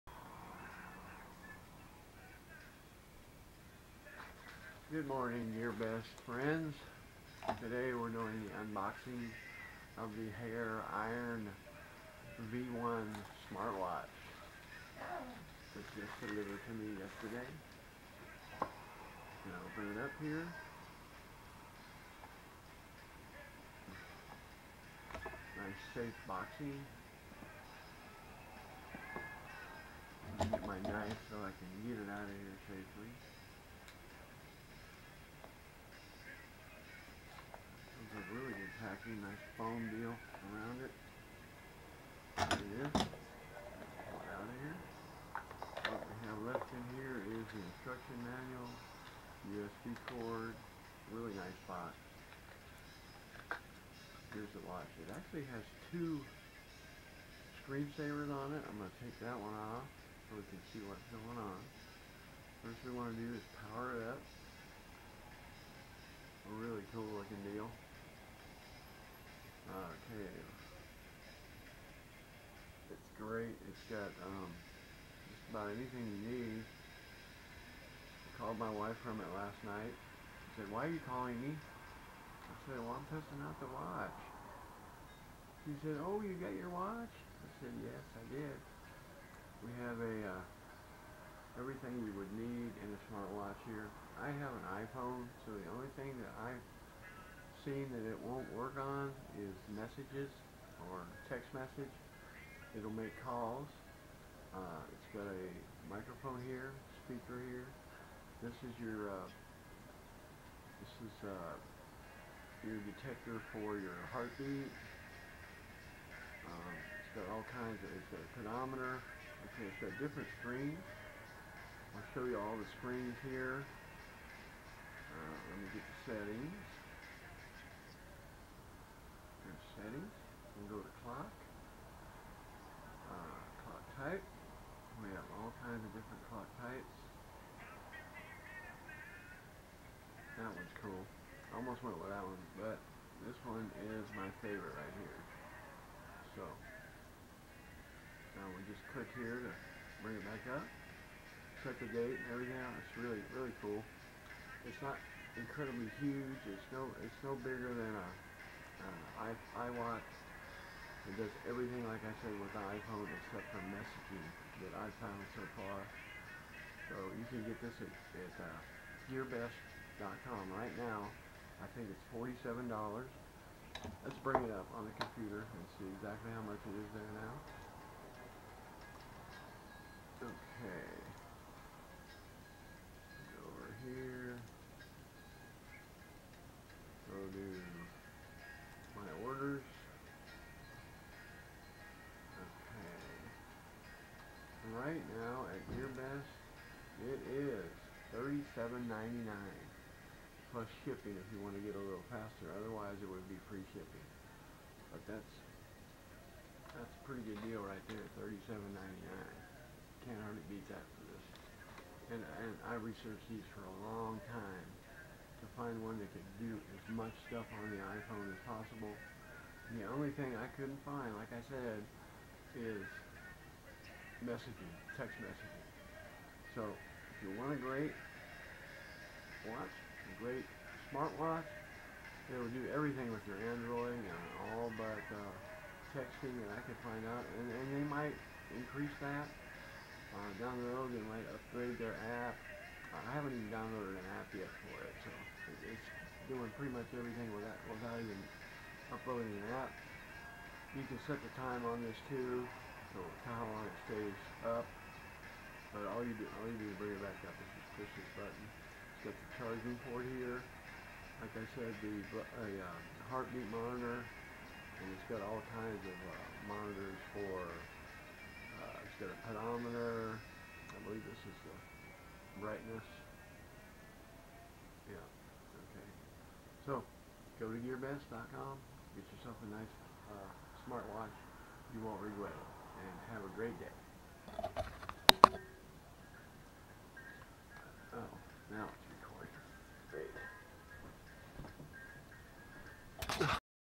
After extensive research, this watch will do upwards of 90% of all functions with my iPhone 6.
for under $40 RIGHT NOW !